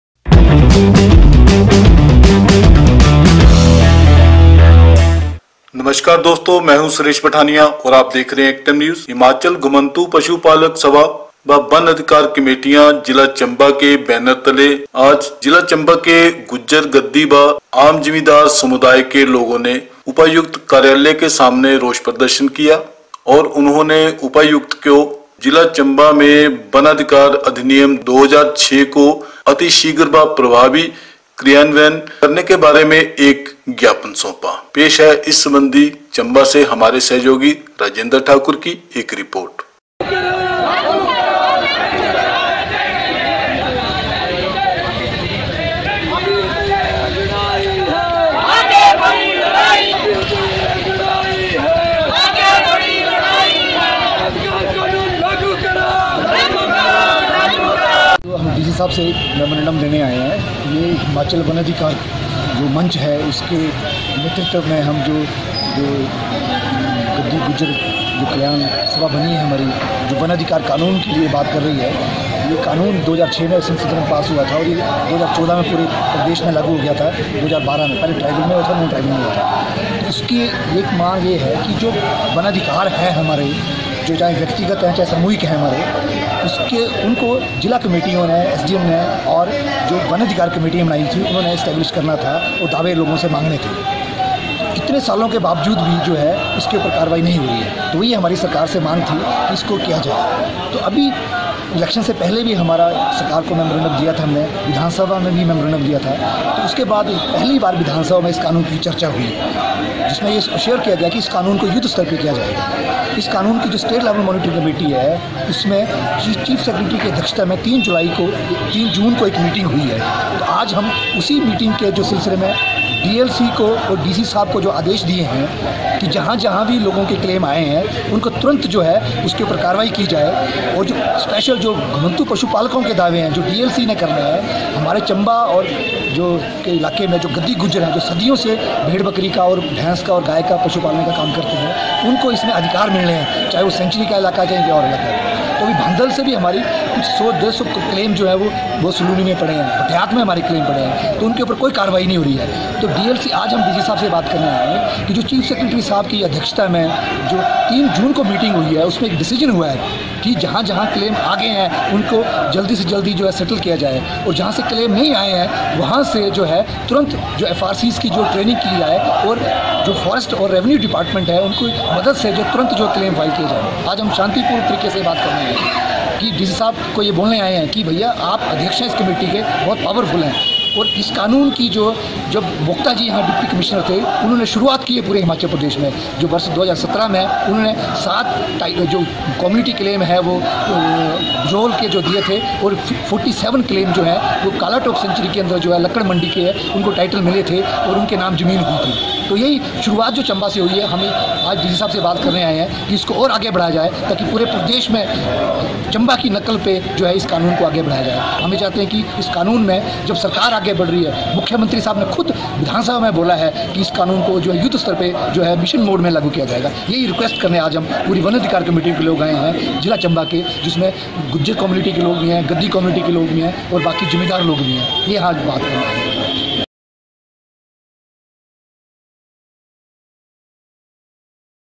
गुज्जर ,गद्दी समुदाय के लोगों का वनाधिकार अधिनियम 2006 के क्रियान्वयन के लिए रोष प्रदर्शन
हिमाचल घुमन्तु पशुपालक सभा व् वन अधिकार कमेटीआं जिला चम्बा के बैनर तले आज जिला चम्बा के गुज्जर ,गद्दी व् आम जिमीदार समुदाय के लोगों ने उपायुक्त कार्यालय के सामने रोष प्रदर्शन किया और उपायुक्त को एक ज्ञापन दिया जिसमें जिला चम्बा में वनाधिकार अधिनियम 2006 को अतिशीघ्र व् प्रभारी क्रियान्वयन करने की मांग की